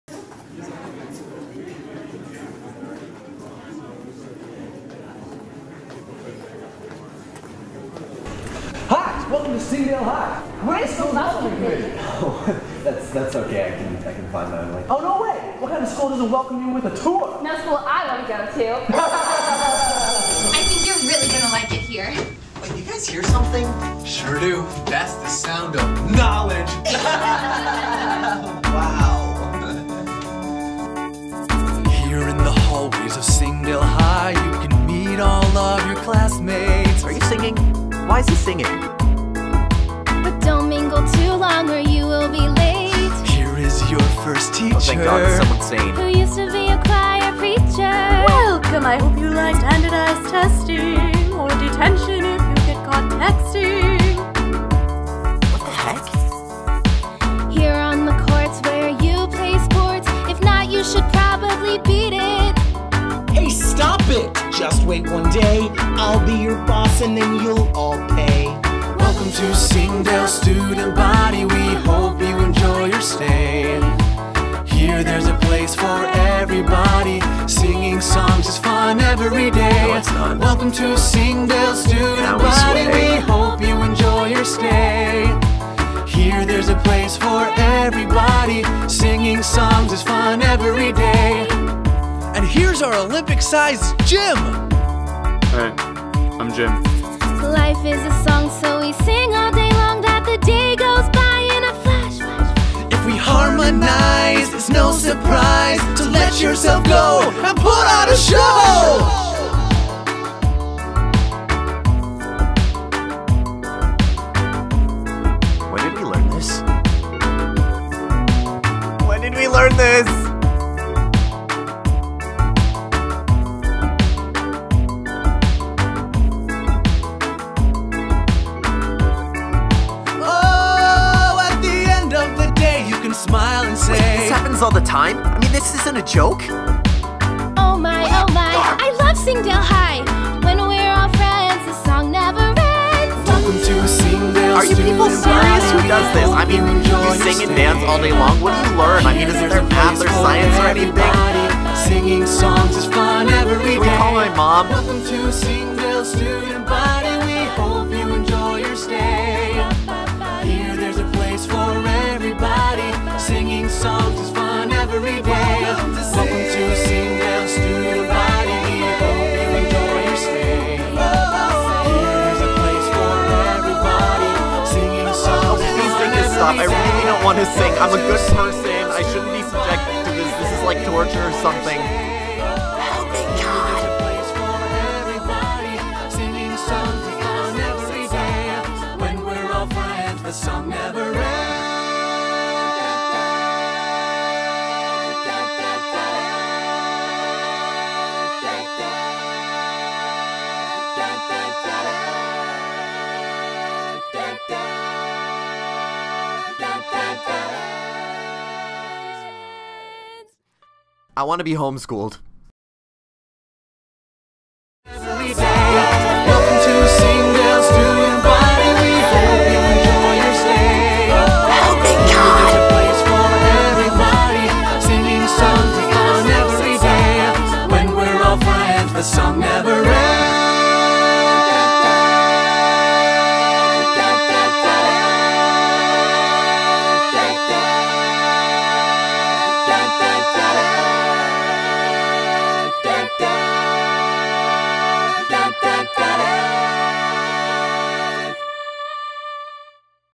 Singdale High
What would happen in real life if you ended up at a High School where people randomly burst into song and dance